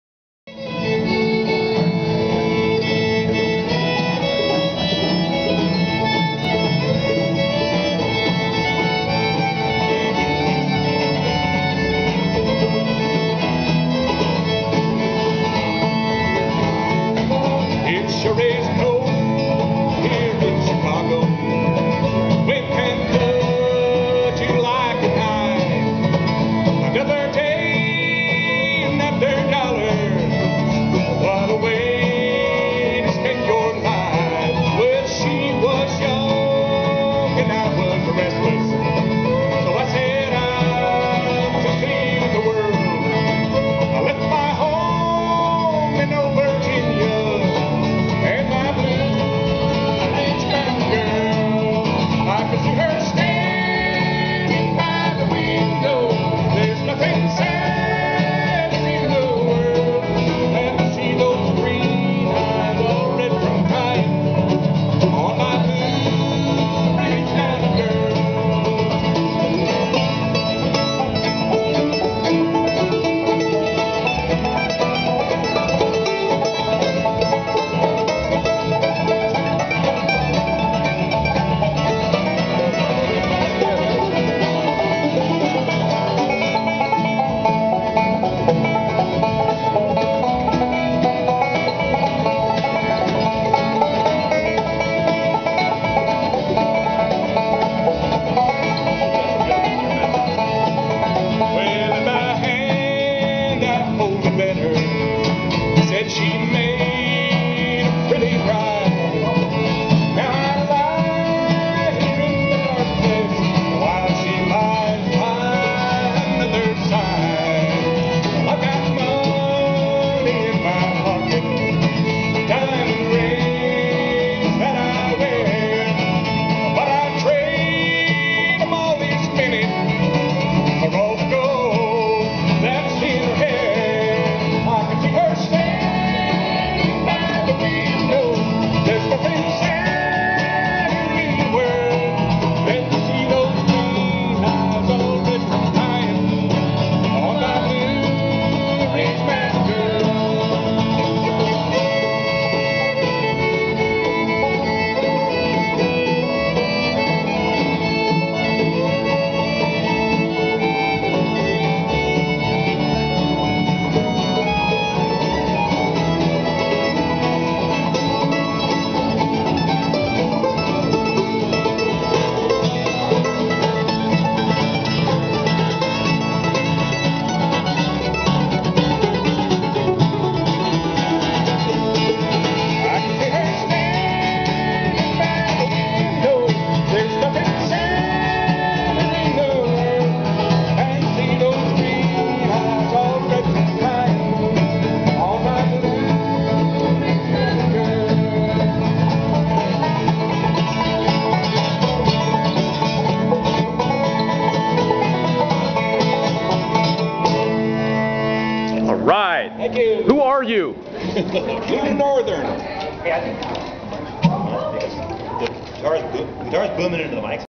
George Ireton and Blue Northern
Redding Sunrise Rotary sponsors "Breaking Down The Walls" drive thru and sit down BBQ to benefit Central Valley High School in Shasta Lake City, CA. " I always thought it would be cool to have a parking lot BBQ business with bluegrass and call it Billy Bob's Park & Pork"! That's cool, George. George Ireton, of Shasta Consulting and Web Design, is quite the noted bluegrass musician, with many a creative idea for networking and good ol community service. All Greater Redding Chamber of Commerce members take note. THIS GUY IS GOOD! We need to get George and the band to do a Chamber funtion some time. I've been to 2 world fairs, and a hog calling, but this is knee slappin, heart warmin bluegrass fun at it's best. Thanks for the great evening, George!    Ken Swanson